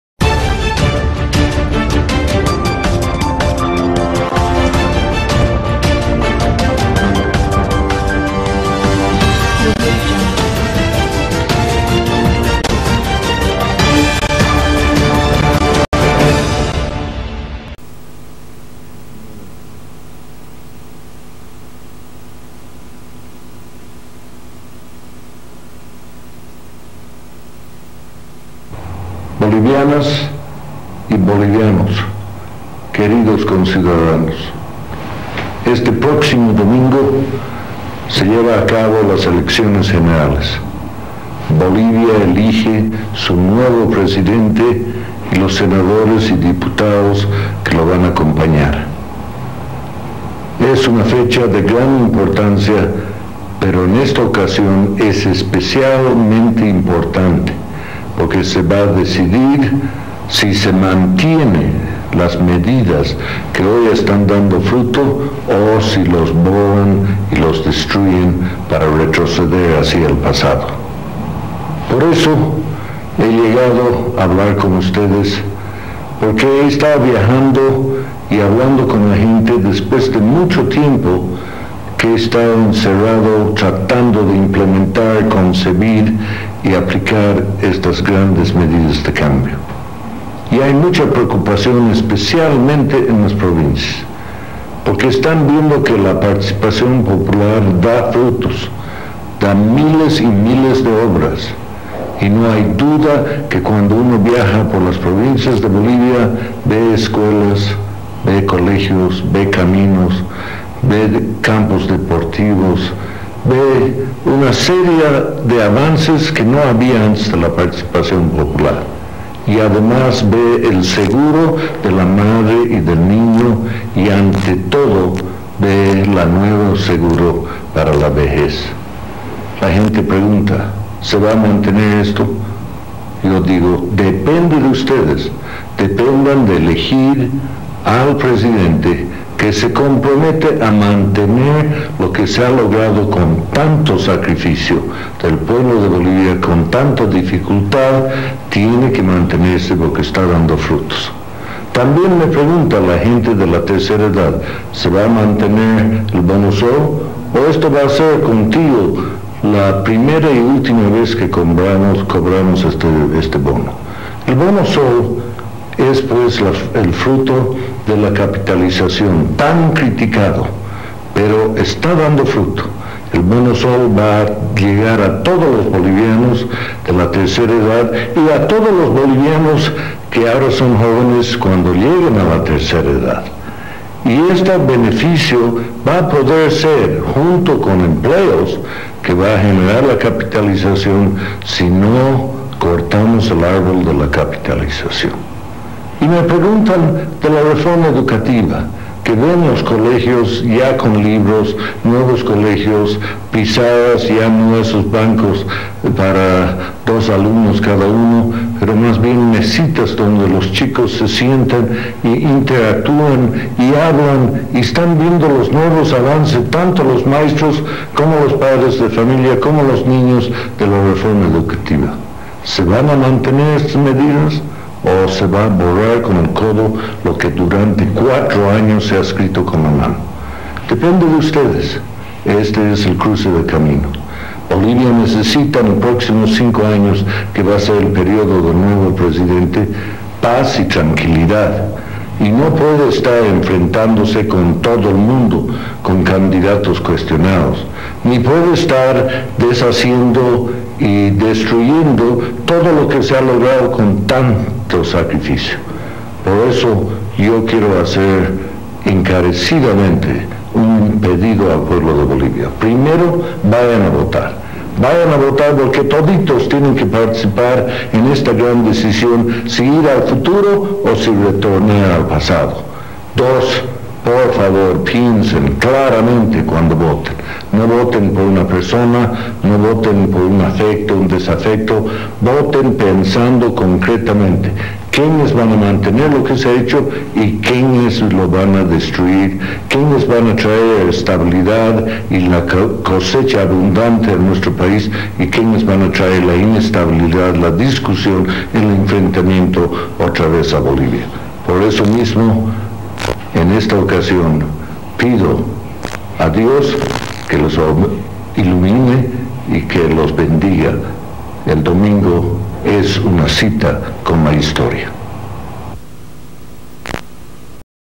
Mensaje Sanchez de Lozada 1997
Mensaje de Gonzalo Sanchez de Lozada un dia antes a la realizacion de elecciones en 1997